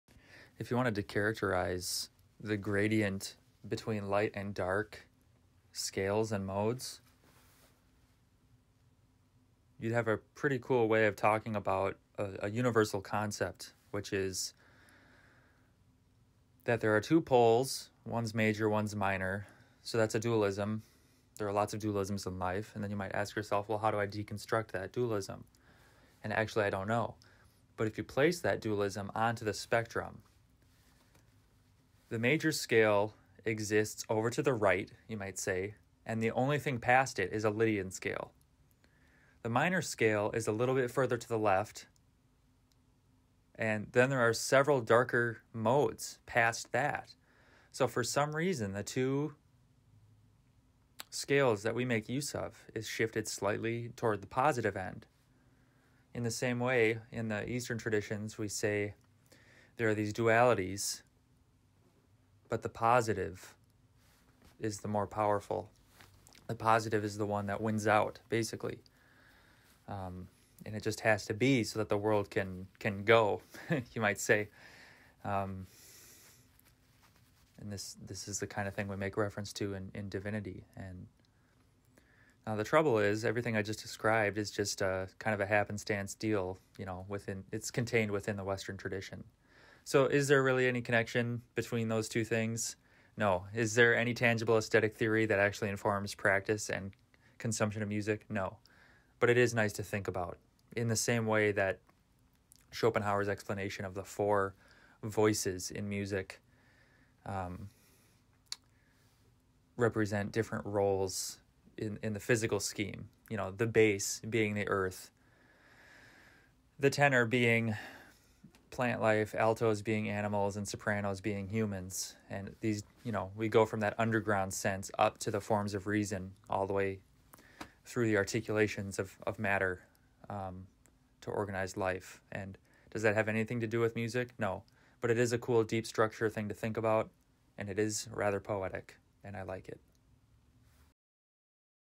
musical modes and spirituality
Quick analysis of modes in terms of duality still containing a bias to the positive.

Also quick discussion of Schopenhauer’s theory of the bass being earth, tenor being plants, altos being animals, and sopranos representing human reason.

These articulations don’t have an effect on our direct musical cognition but they’re nice ideas.